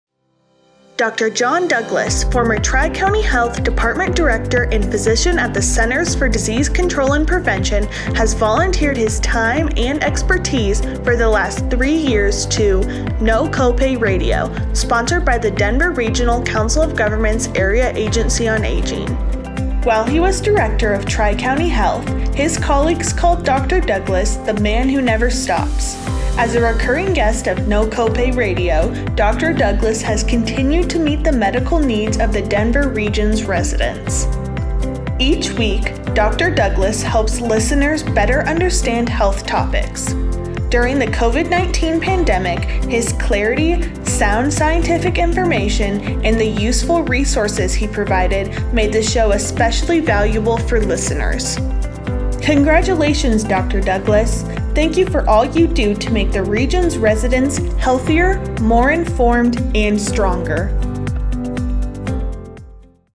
2023 Distinguished Service Award: Dr. John Douglas
Former Tri-County Health Department director and physician at the Centers for Disease Control and Prevention, Dr. Douglas has worked with DRCOG's Area Agency on Aging for the last three years on their radio show, "No Copay Radio." Thanks to his expertise and sound scientific information, Denver region residents have been able to stay current on the COVID-19 pandemic.